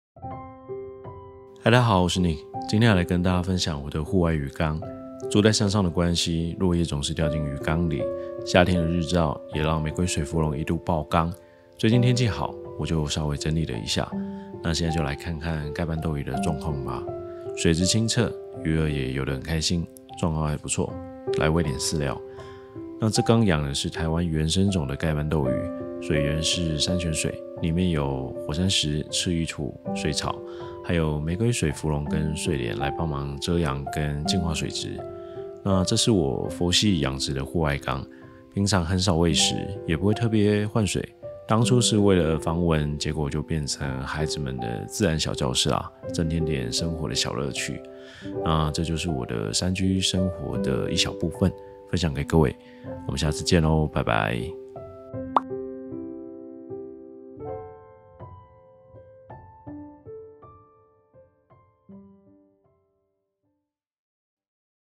山居生活：分享蓋斑鬥魚戶外缸近況
Hi，大家好，我是Nick！
今天跟大家分享我的戶外魚缸。

住在山上，落葉總是掉進魚缸，夏天的日照也讓玫瑰水芙蓉一度爆缸。最近天氣好，我就整理了一下，來看看蓋斑鬥魚的狀況～水質清澈，鬥魚游得很開心，狀態不錯，餵點飼料吧！

這缸養的是台灣原生種蓋斑鬥魚，水源是山泉水，裡面有火山石、赤玉土、水草，還有玫瑰水芙蓉和睡蓮來幫忙遮陽和淨化水質。

這是我佛系養殖的戶外缸，平常很少餵食，也不特別換水。當初是為了防蚊，結果變成了孩子們的自然小教室，增添了生活的小樂趣。

這是我的山居生活，分享給各位
我們下次見，拜拜！